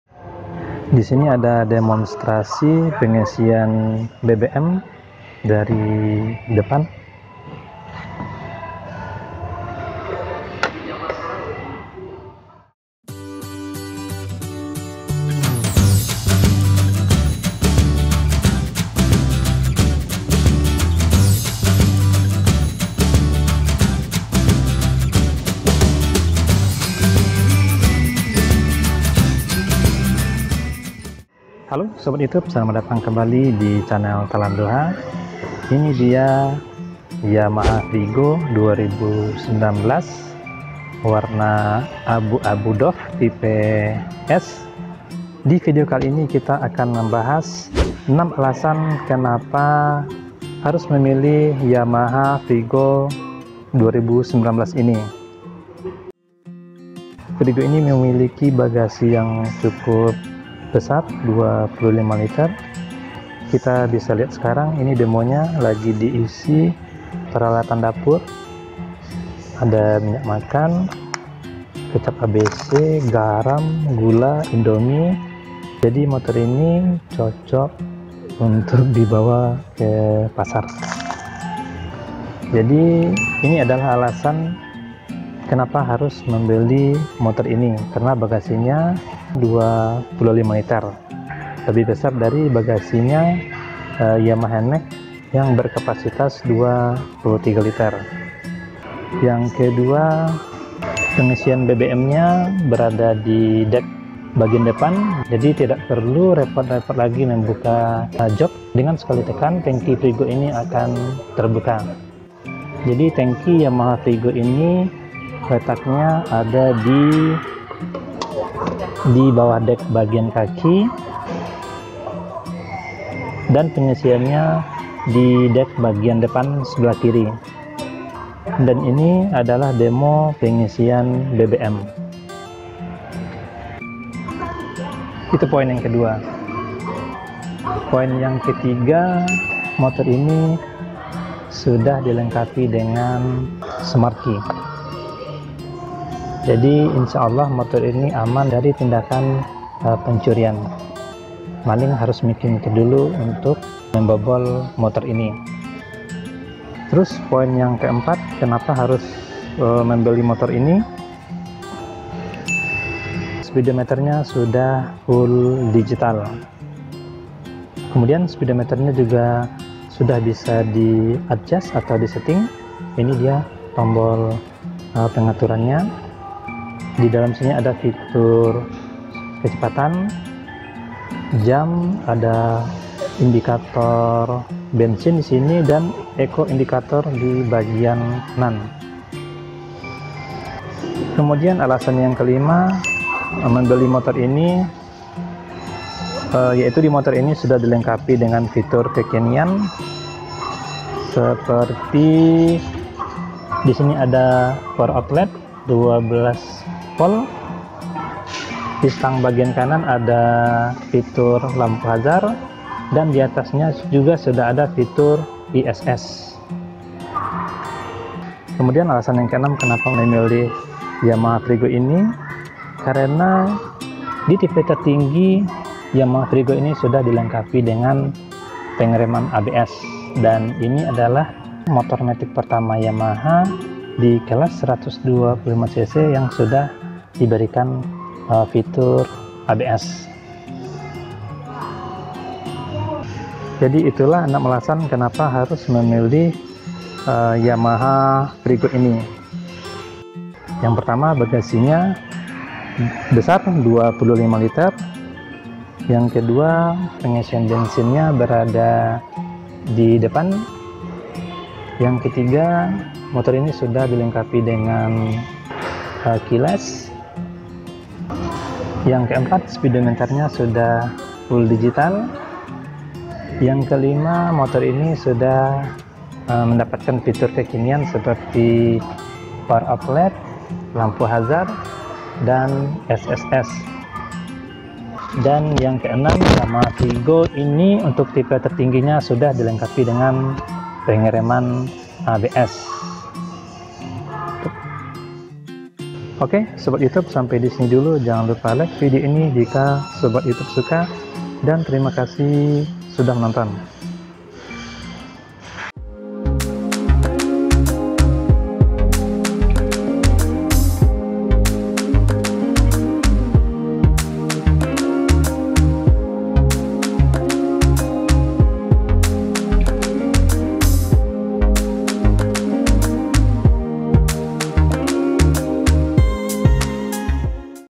6 Alasan Kenapa Memilih Yamaha Freego S Daripada Genio, Scoopy dan Vario 125
Divideo Kali Ini Kita Akan Membahas, Kenapa Harus Memilih Yamaha Freego? Yuk Tonton Videonya Sampai Selesai